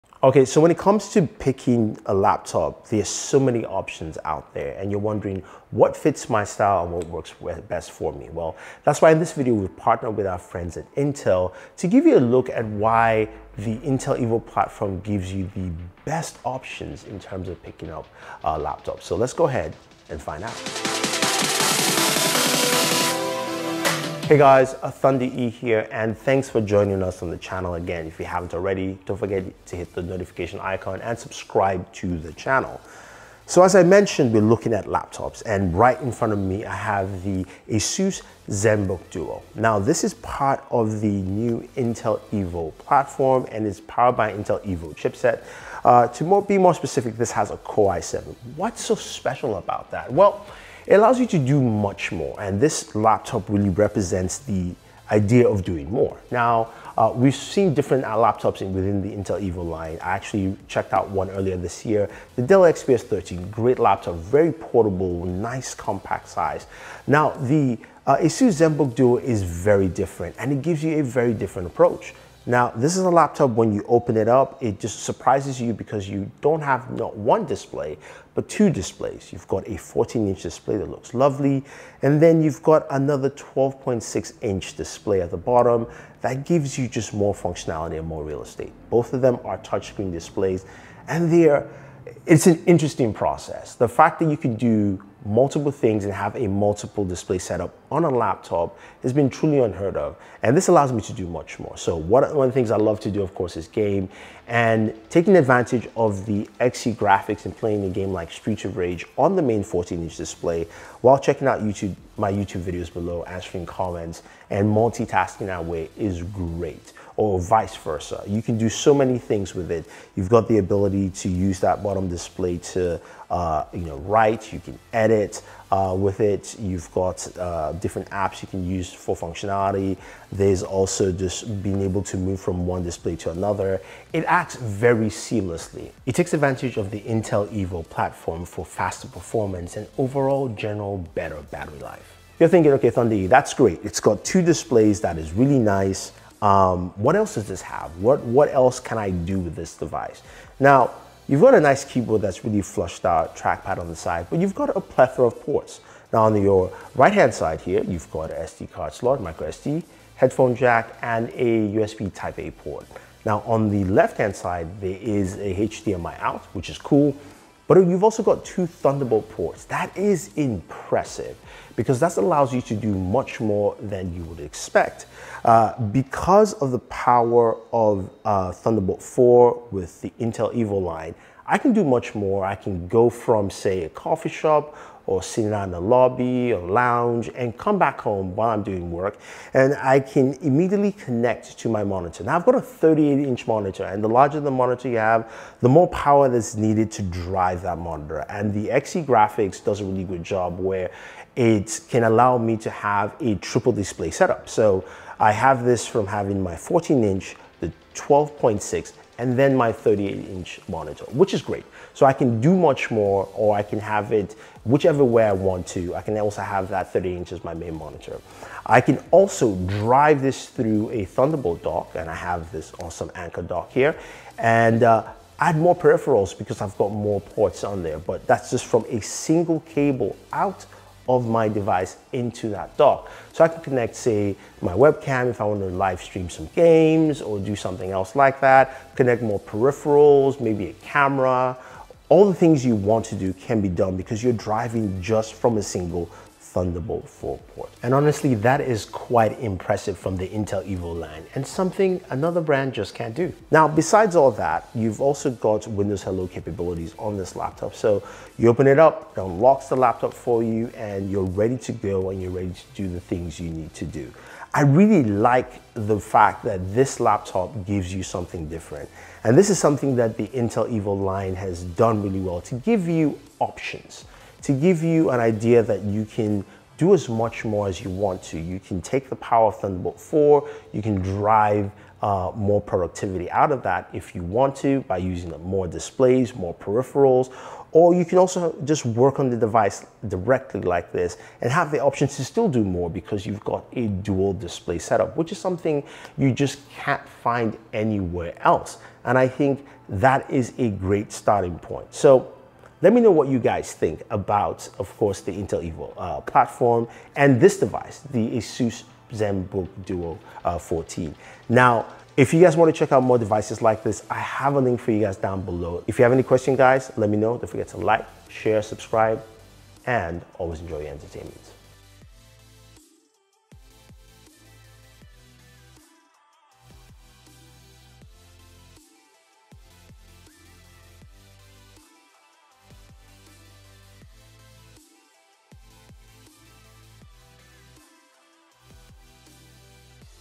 Your next Laptop NEEDS this!
Your next laptop needs a few key features which can be found within INTEL's new Evo platform. Offering a slew of laptop and connectivity options through Thunderbolt that can only be offered here. Let take a quick look at the new Asus Zenbook duo 14.

Asus Zebook Duo 14: v
Anker Thunderbolt 4 dock